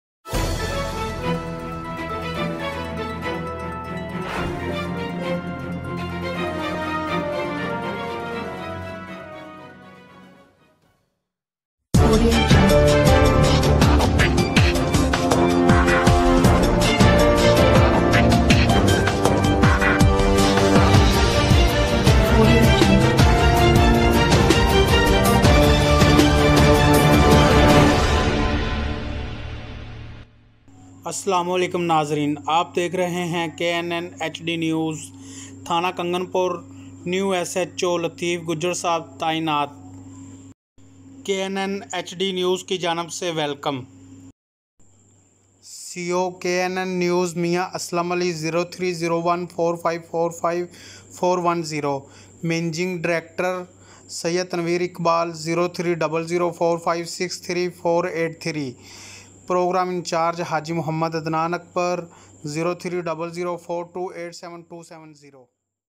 Welcome from KNN News stationed at Kunganpur New SHO latif Gujjar Sahib
Welcome from KNN News stationed at Kunganpur New SHO latif Gujjar Sahib KNN HD NEWS KARWAN NEWS KANGAN PUR تھانہ کنگن پور نیو ایس ایچ اولطیف گجر صاحب تعینات کے این این نیوز کی جانب سے ویلکم